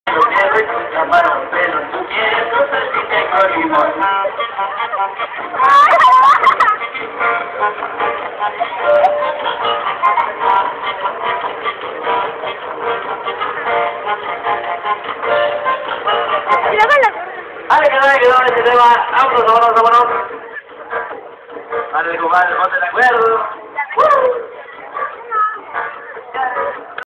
dia de carnaval
es en el pueblo de Santa Rita Tlahuapan  un domingo muy divertido!!